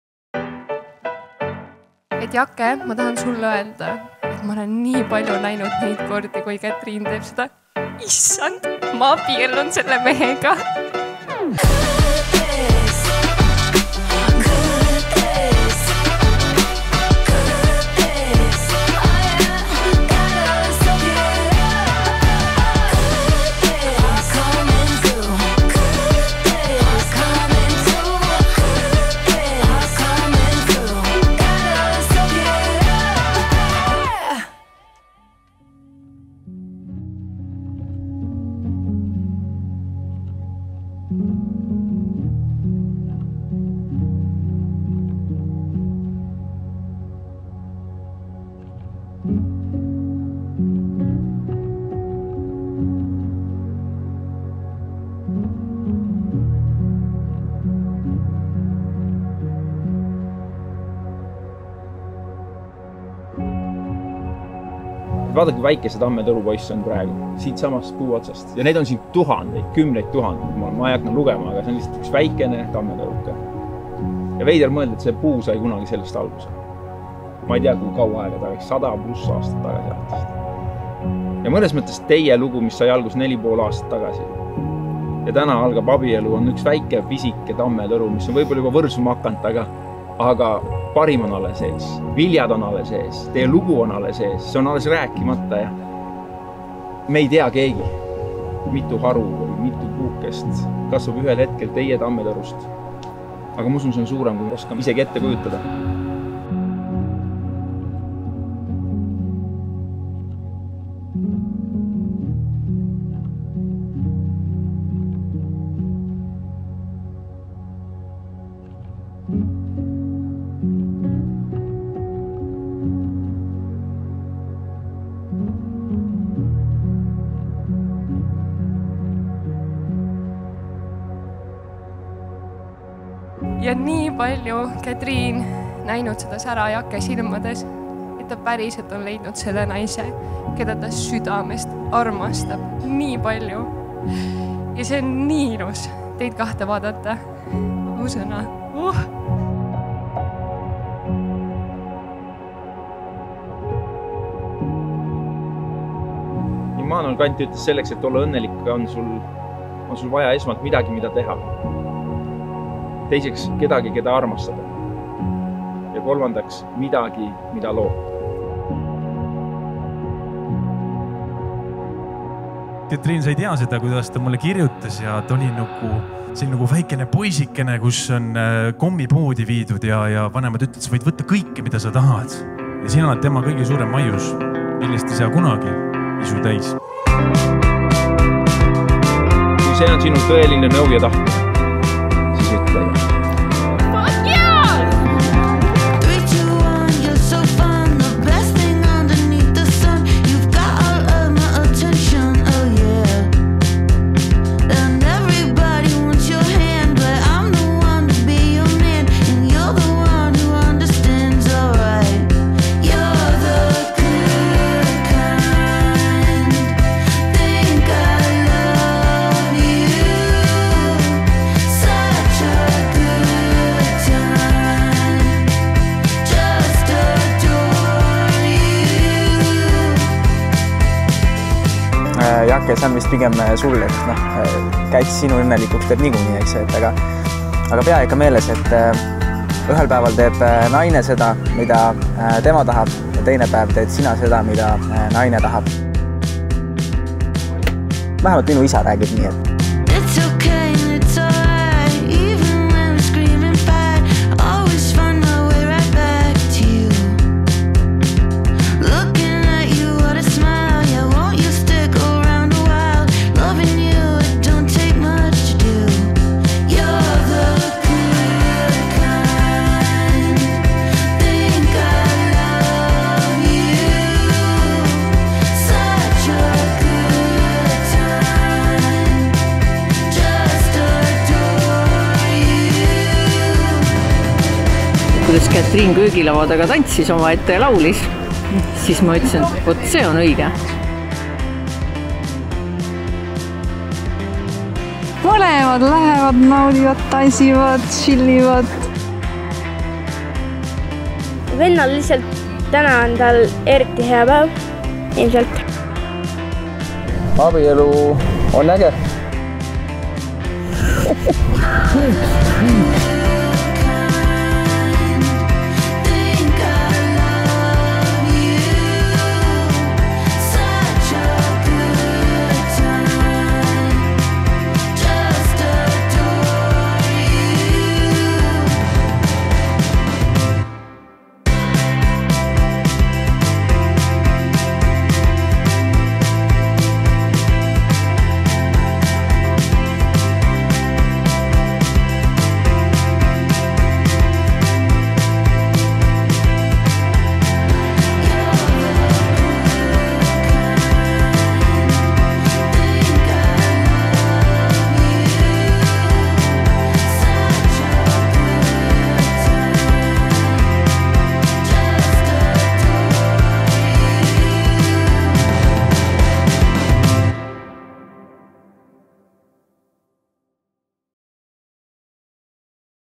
Kätriin & Jake lühike pulmavideo | Short Wedding Video
Kui ülilahe, andekas ja tuntud fotograaf Jake (jakefarra.com) otsustas abielluda imelise Kätriiniga ning nad soovisid oma pulmapäevast mälestuseks ka pulmafilmi, mille teostumiseks sain anda oma südame ja energia, siis tegi see mind algul omajagu kohmetuks küll.
Äge, milline päev, inimesed ja koht, kus pulmapidu teha - oma koduhoovis, keset askeldavat linna, oma väikeses turvalises ja õdusas maailmas parimate sõprade keskel.

MUAH: Maret Ubaleht
Wedding coordination and decoration: Sigrid Kullerkupp (GlobeFlower)
MC: Peep-Ain Saar
Photography: Stina Kase
Videography: Urmo Lamp and Taivo MIller | Urmo Lamp Films
Host: Kristjan Lüüs
Bus: 1Buss
Band1: Välk
Band2: Dance-O-Phones
Bar: Butterfly Lounge
Catering: Põhjala Catering
Cake: Heidi Park
Ice cream:  Pehmik

MB01UCXOEAYHH8R